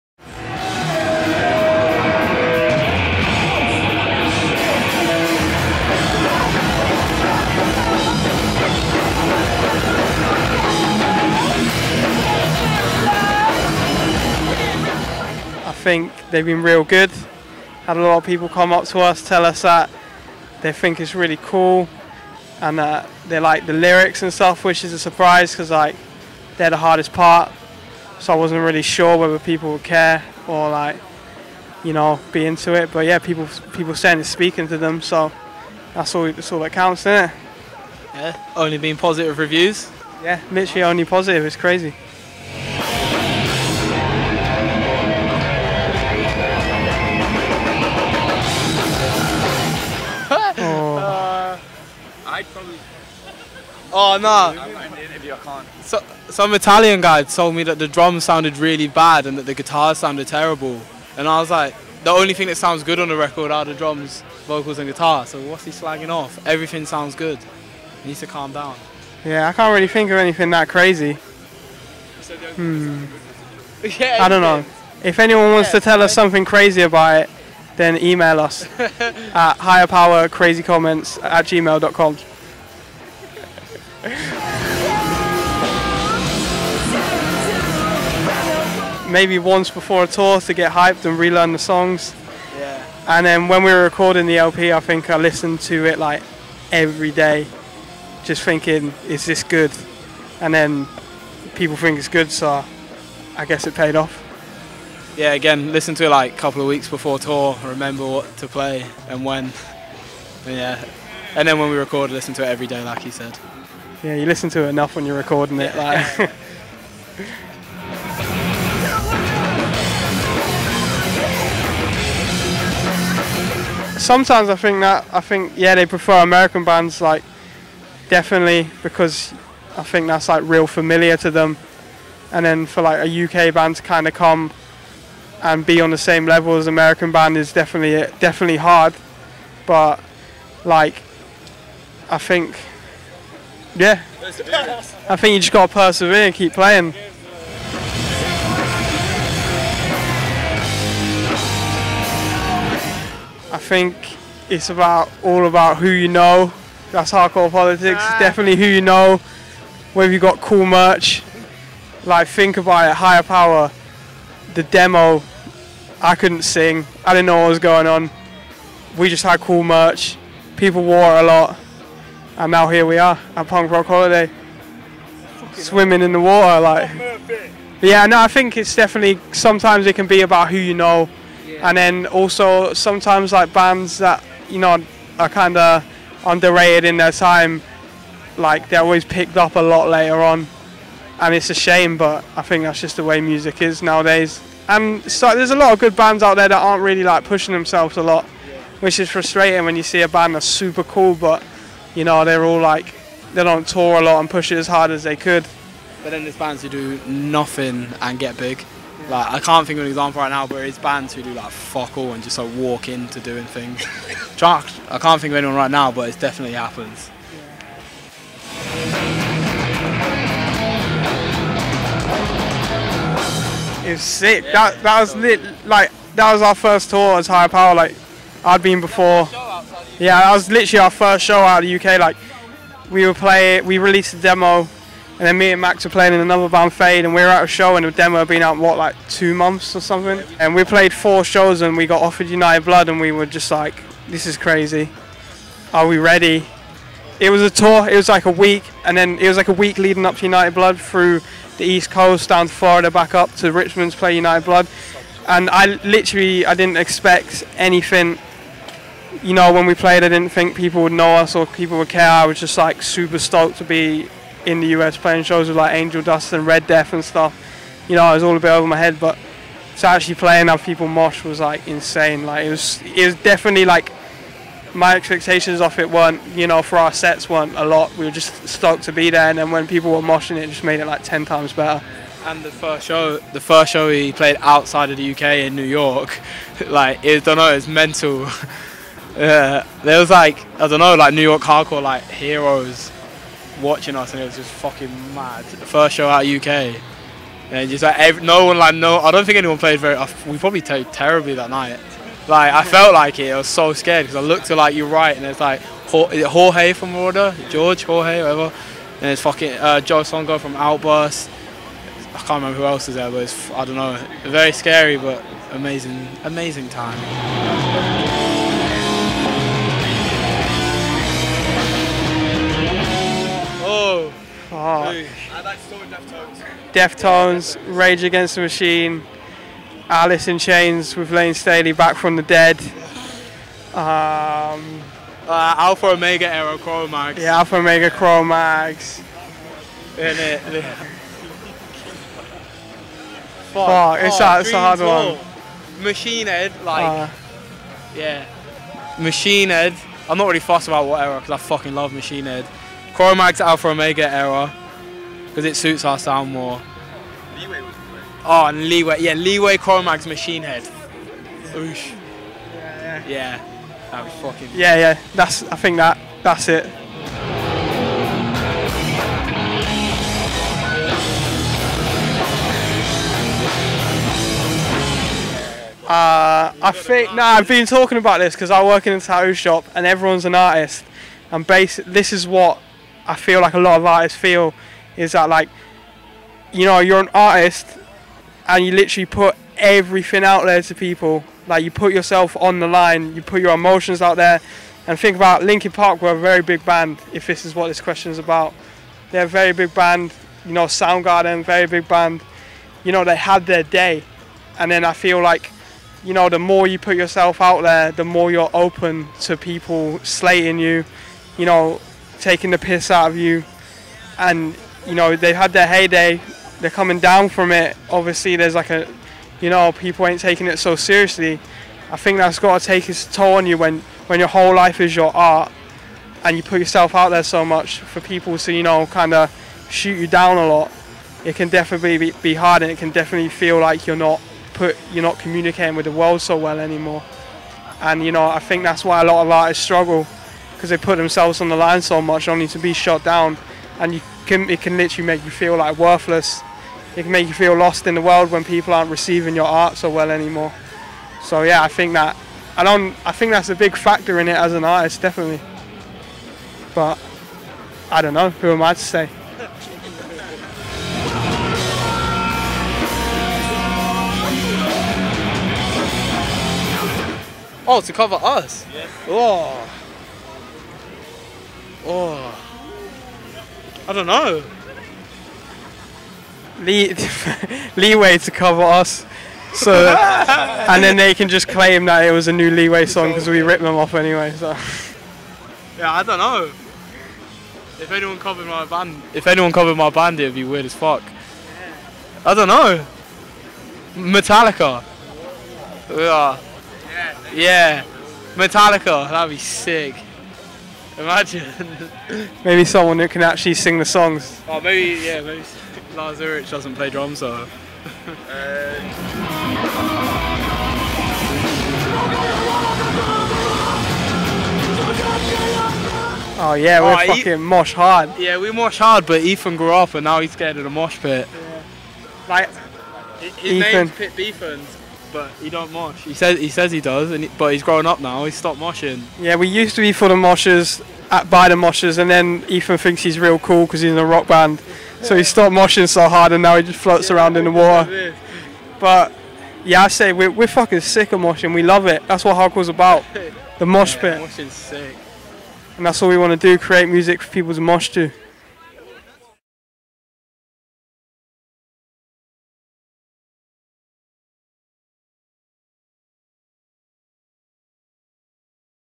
HIGHER POWER / Interview / Slovenia, 09.08.2017
An interview with Jimmy and Alex of UK hardcore band Higher Power.

00:11 - What are the reactions to the new album?
00:43 - What's the most absurd comment you've heard/read about the new album?
01:26 - How often do you listen to HIGHER POWER at home?
01:58 - Do you think European fans prefer American hardcore bands over European? Is it harder for European bands to make it on the scene?
02:30 - Why some great bands remain unknown and unpopular, and some, which are not so good, become famous?
03:39 - What was your stay in the US like?
05:21 - Dream tour?
06:10 - Why do you think famous rock stars commit suicide?
07:42 - Which band would you like to make a cover of your song?
08:31 - It says: "For the moshers, by the moshers" on your facebook page. Are you really moshers?

Live footages taken at PUNK ROCK HOLIDAY Festival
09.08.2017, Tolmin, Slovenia

Camera/Edit: Vasse